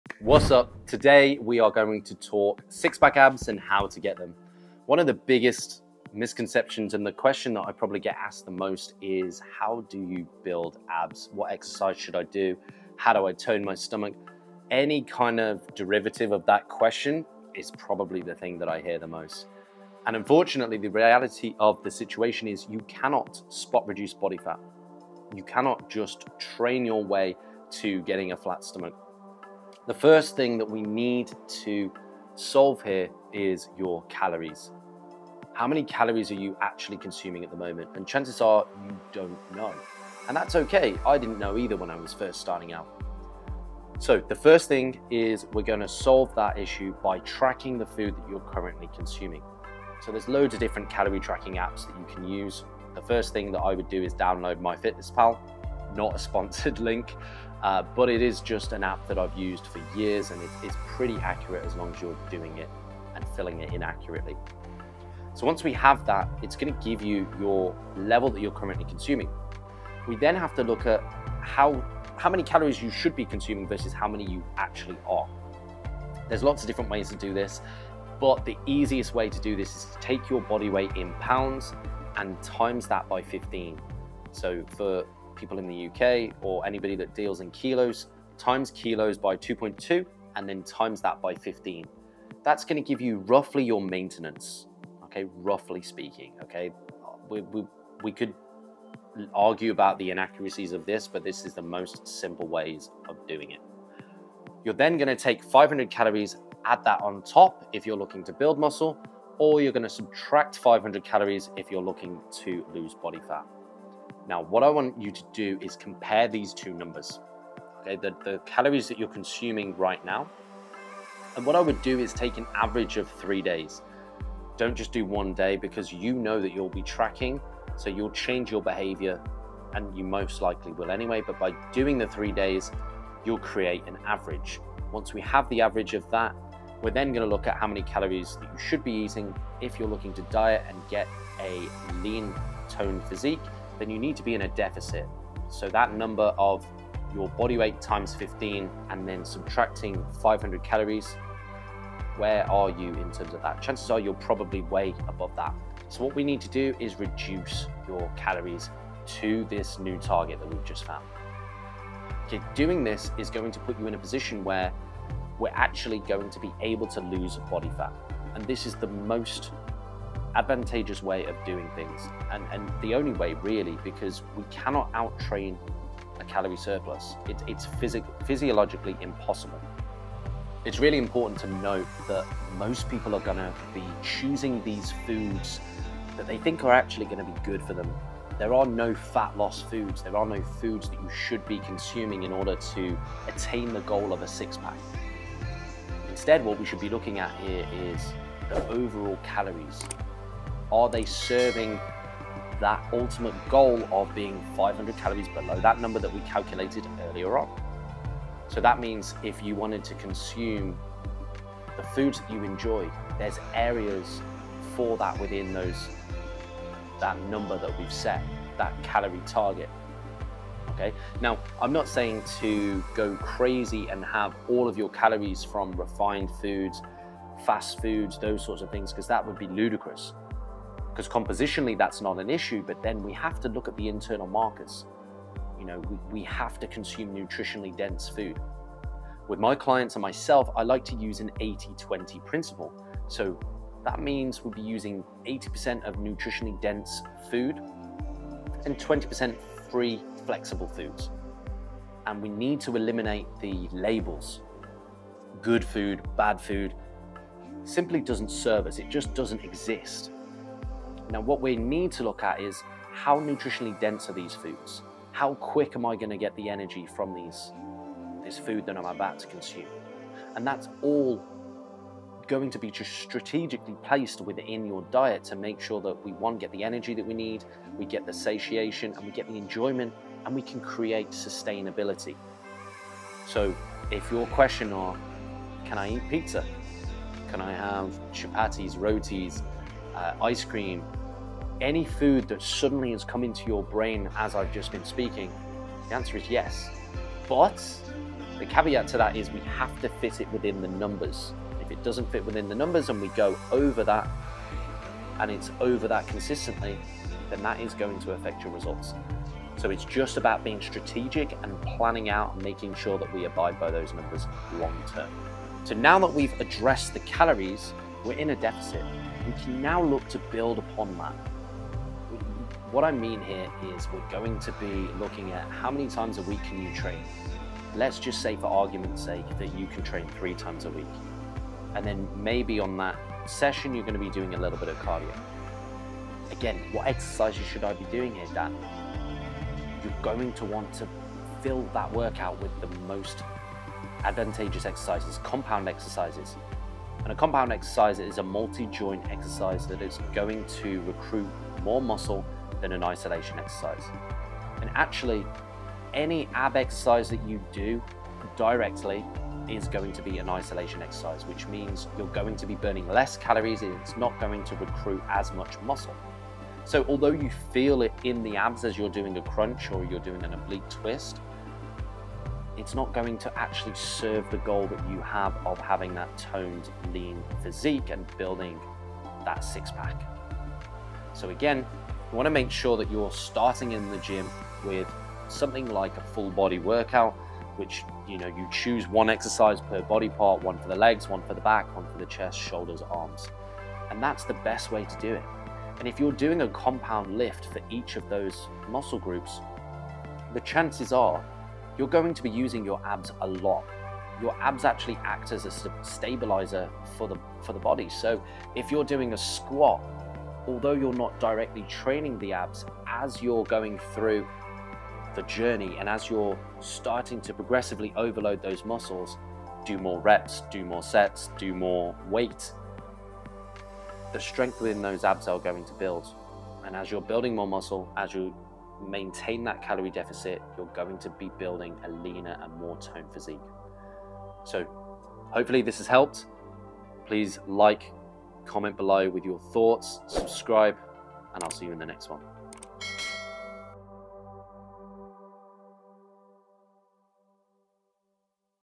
How to get sick pack abs?
This video will help you to focus on the most important factors that will enable you to achieve six-pack abs